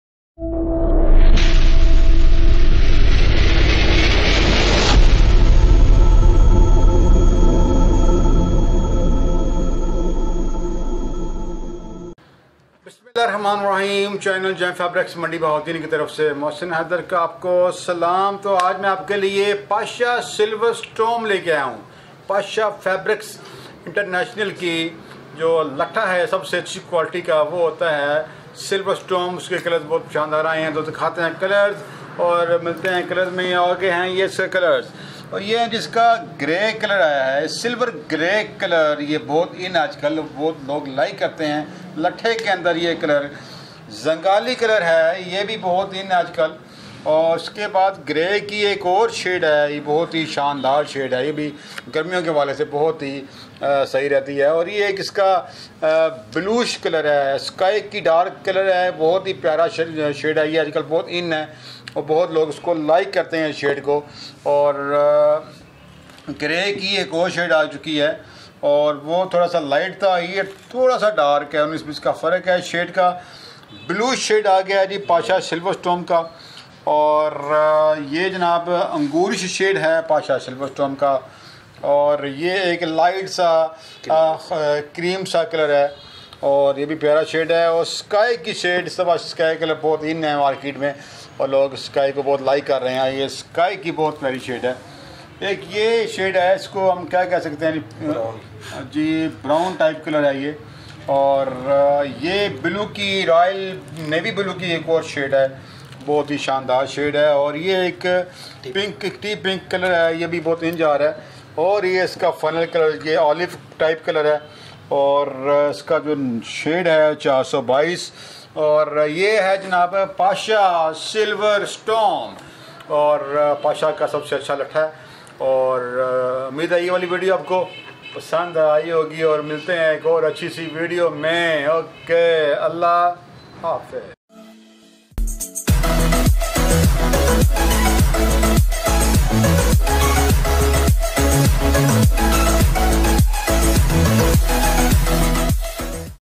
PASHA SILVER STROM LATAH
The fabric that flashes bright sunshine. This is the brightest latha in the world. Latha is normal, household cotton. Because of this quality this is the most popular item of the HOUSE OF PASHA.

Silver Storm has 156 rating on the whiteness index where maximum is 160.